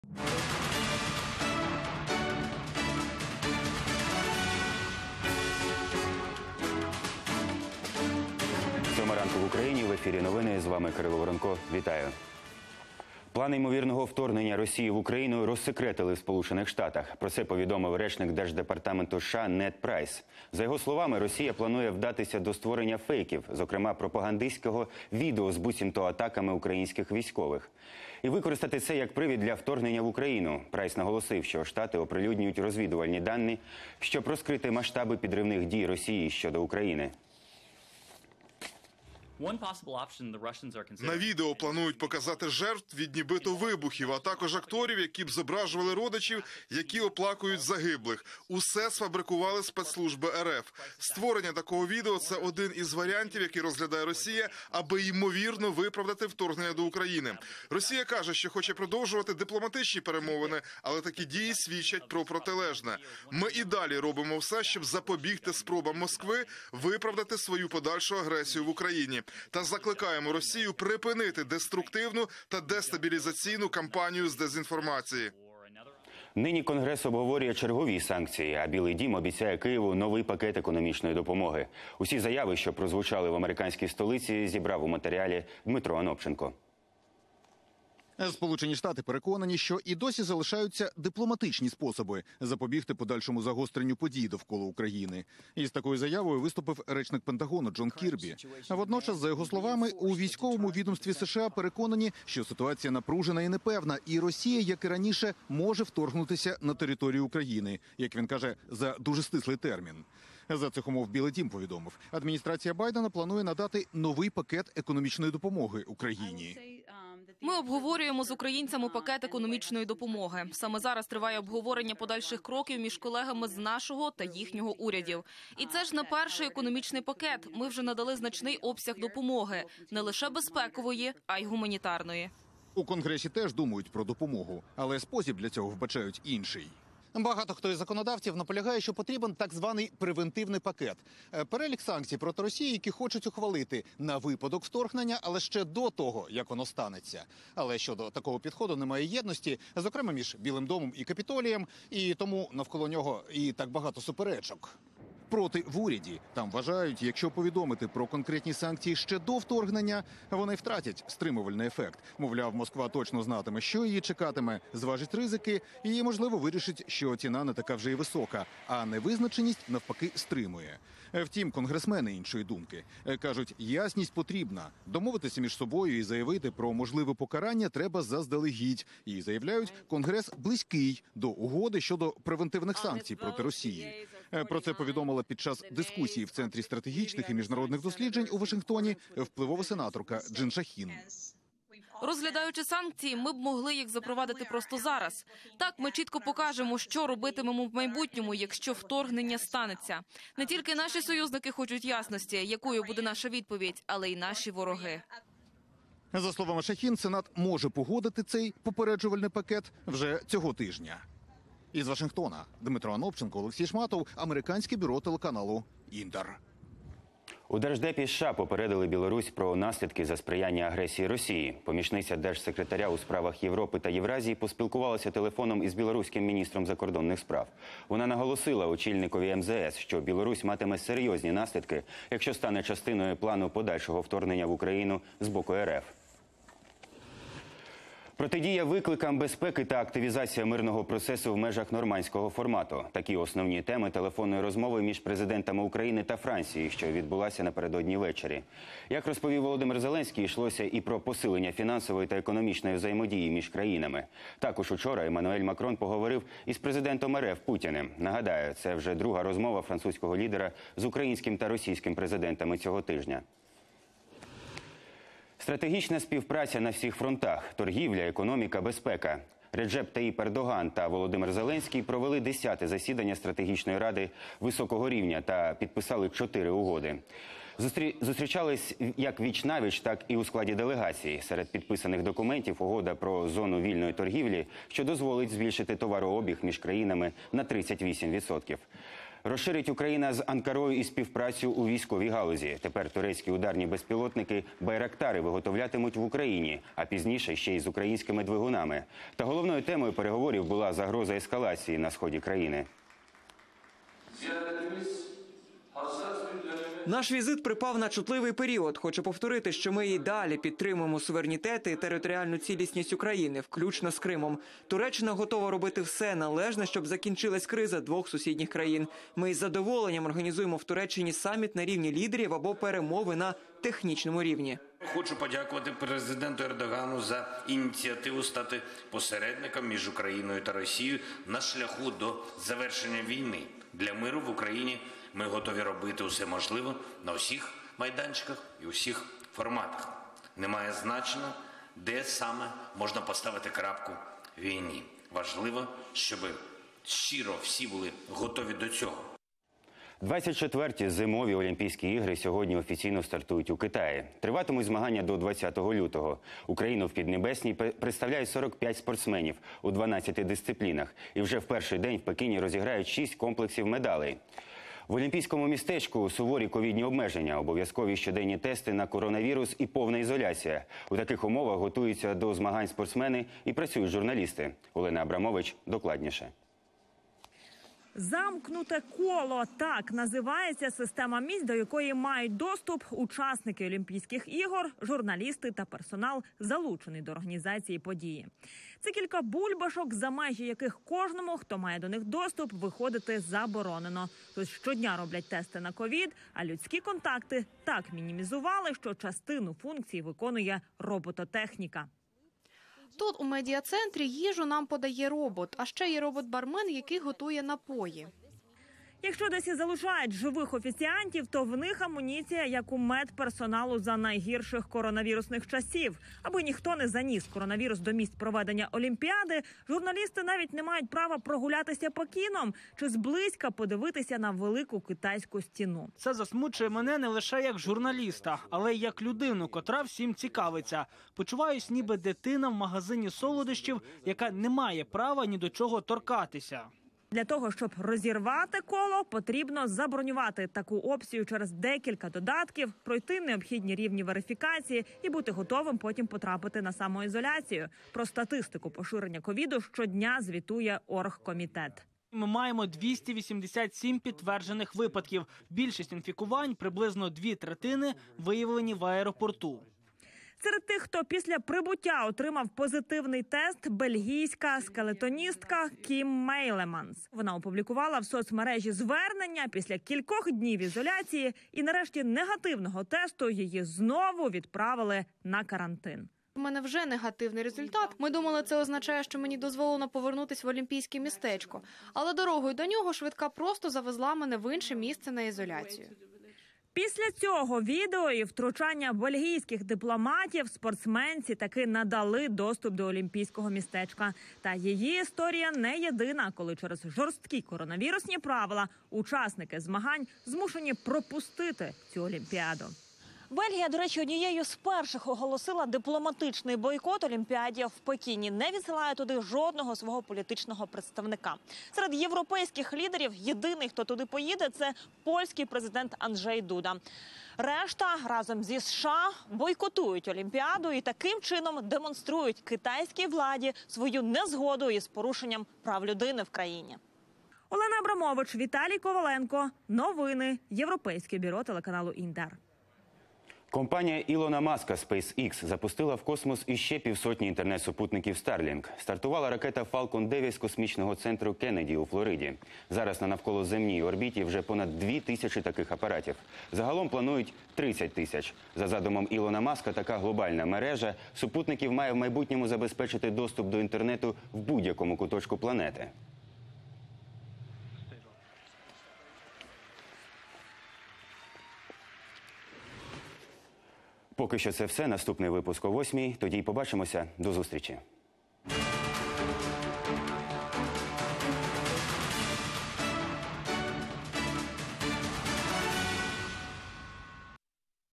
Новости 07:00, выпуск за 4 февраля 2022 года
Дивіться у випуску:
У США розсекретили план імовірного вторгнення Росії в Україну.
Адміністрація Байдена планує надати новий пакет економічної допомоги Україні.
Президенти України та Франції обговорили активізацію мирного процесу в межах "нормандського формату".